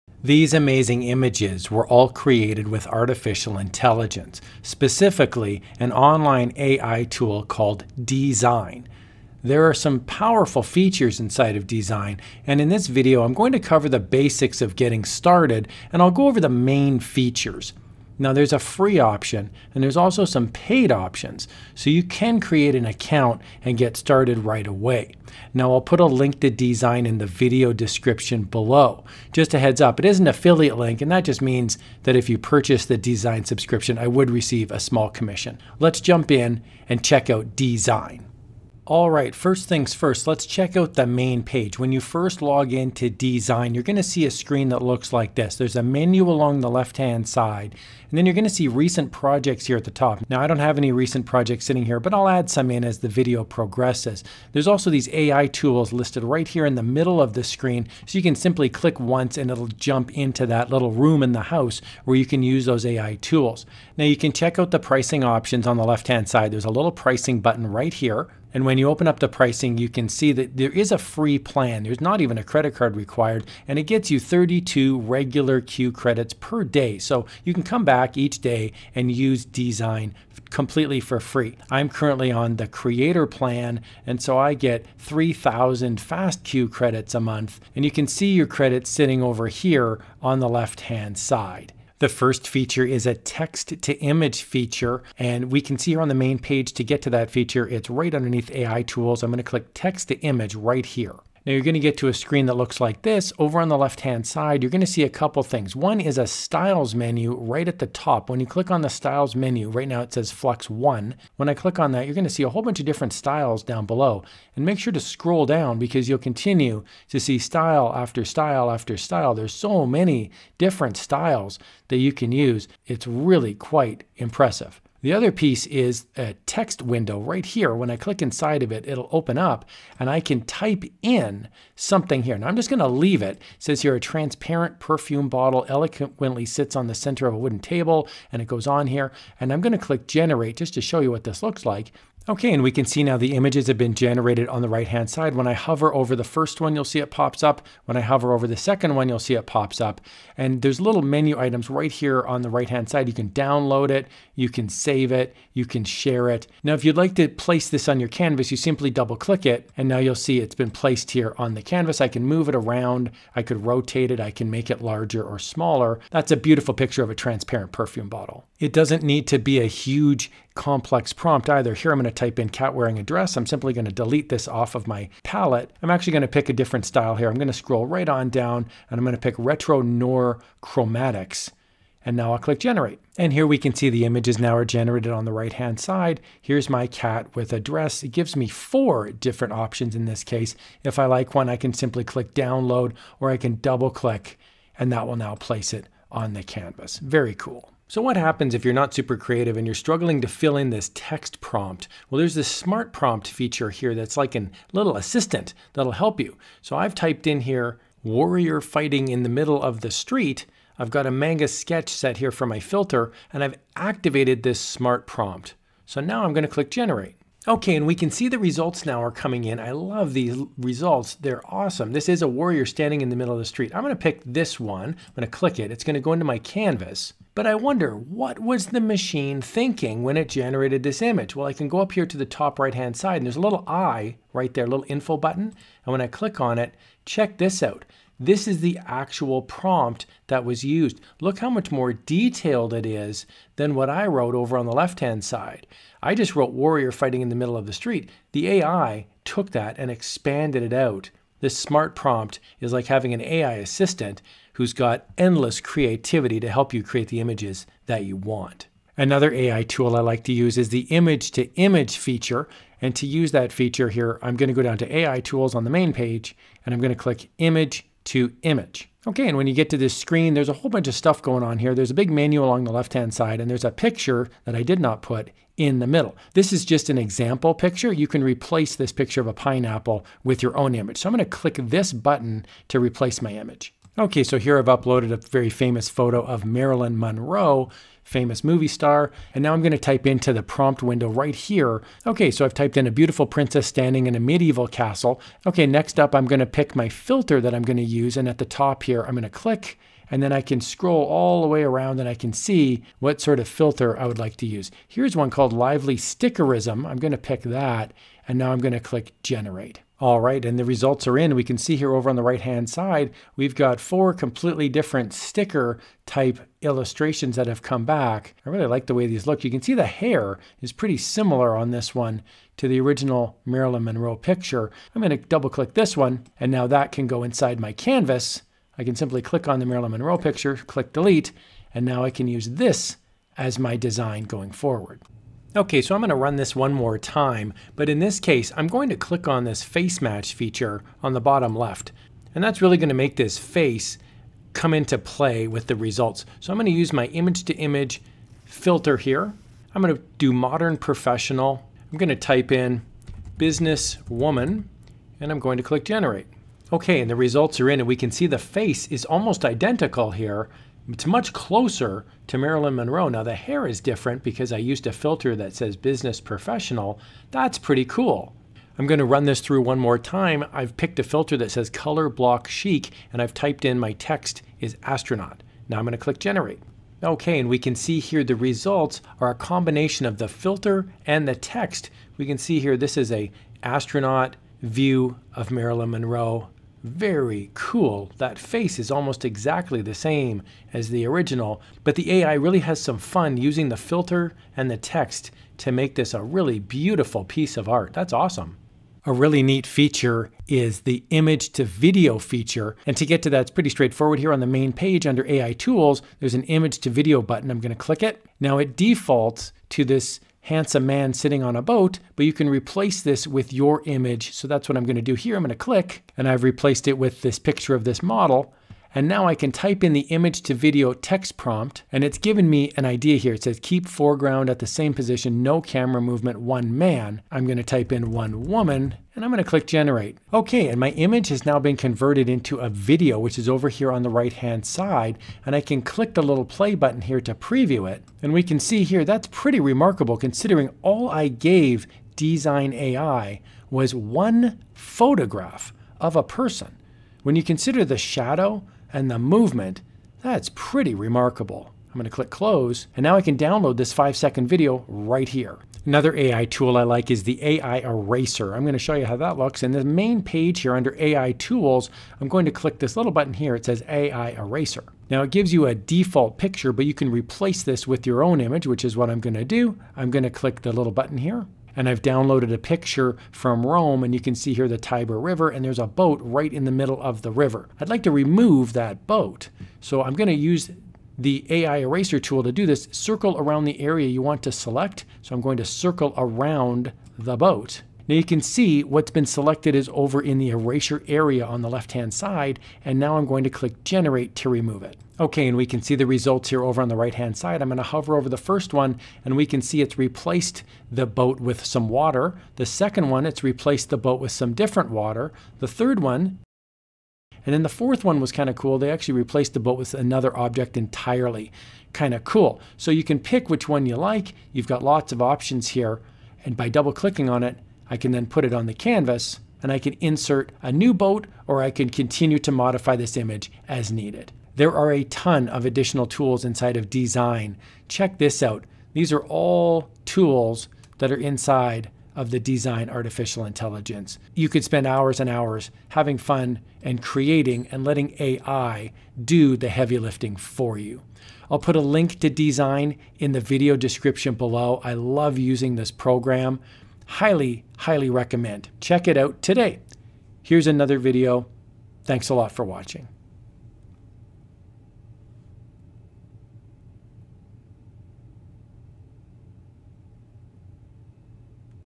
DZINE Beginner's Guide (AI Tool Overview Including Image-to-Video Feature)
There is a great AI tool called DZINE that features text to image, image to image and even image to video! In this video, I cover the basics - how to use the menus and how to create stunning images and videos using the DZINE artificial intelligence. Using DZINE, you can create great-looking artwork and modify images. Start creating today!